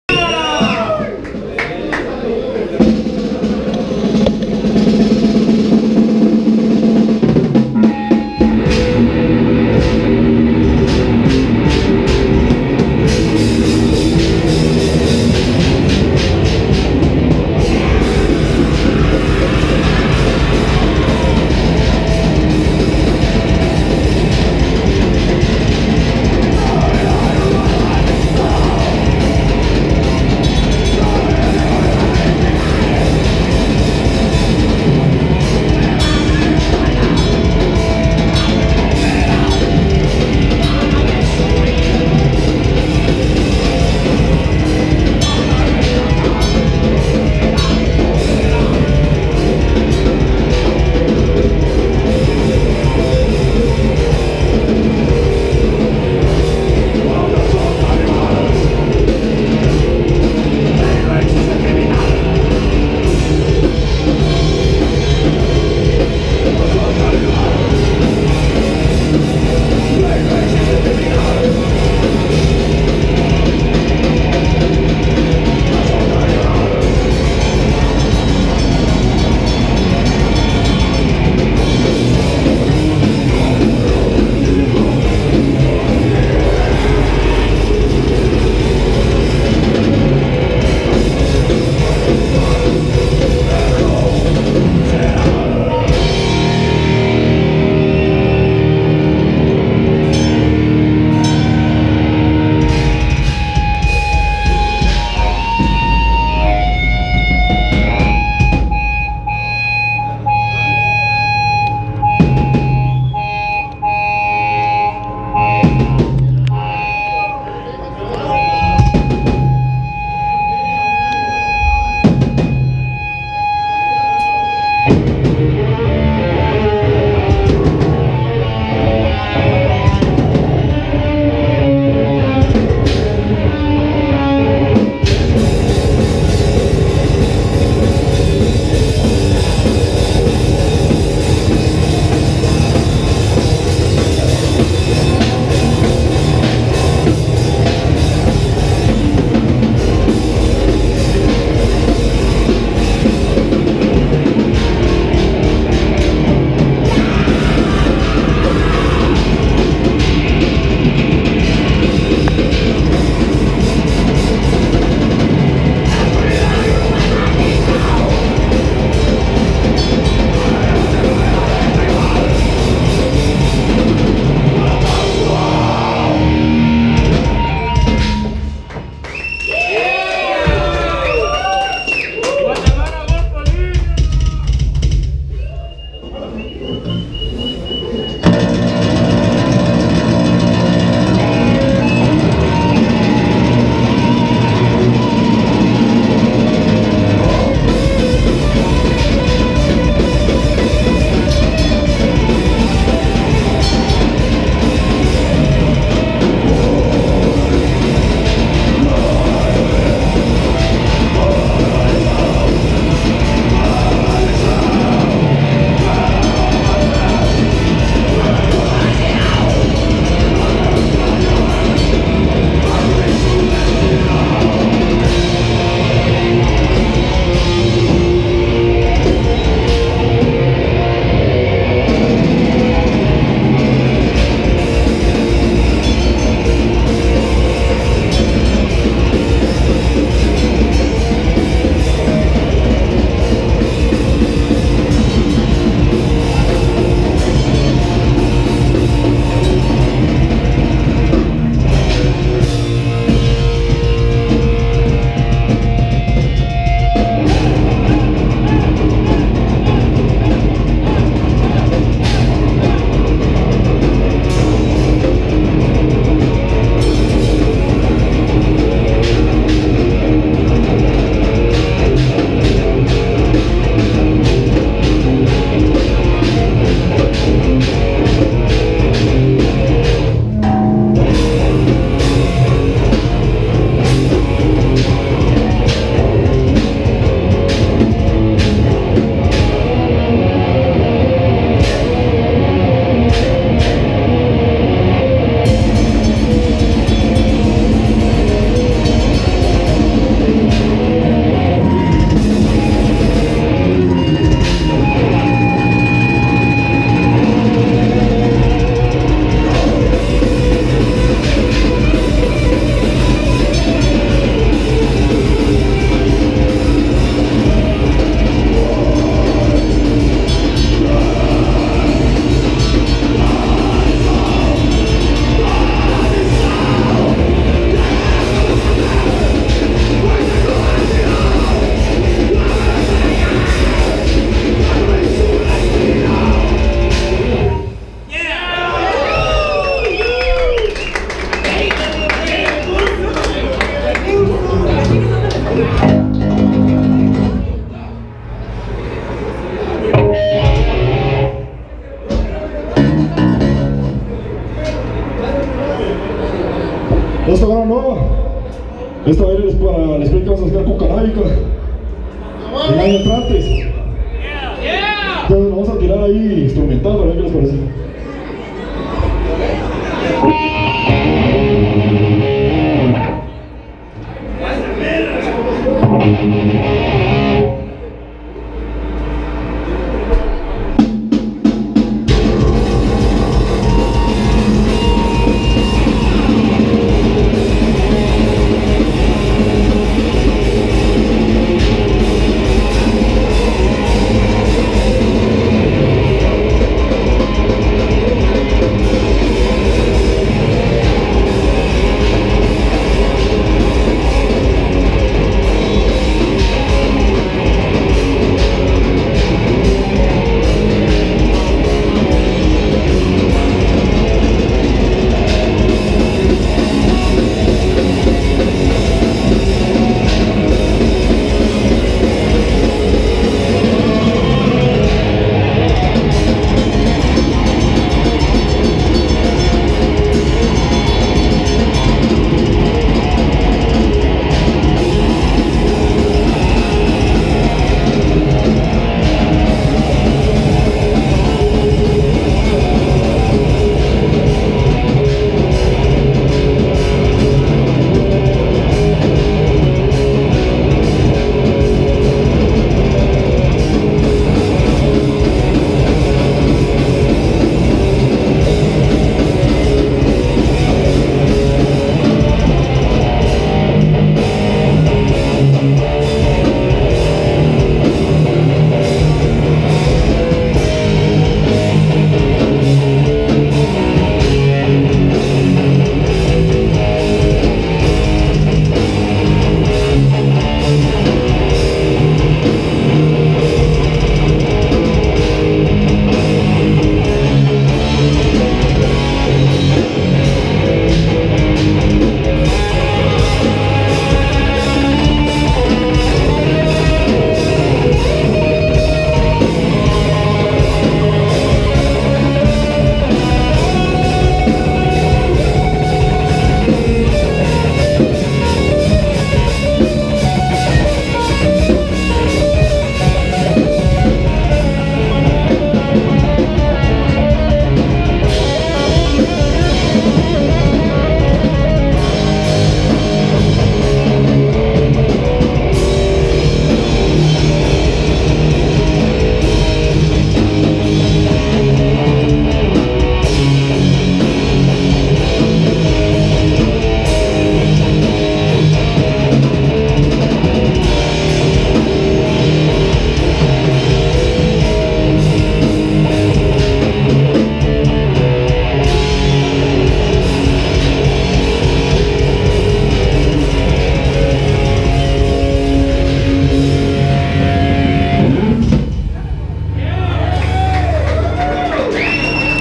NEUROTÓXICO DIC/22/2018 GOPR0002
NEUROTÓXICO  DIC/22/2018  en Rocker's Place, Festival Thrash Infest, Ciudad de Guatemala.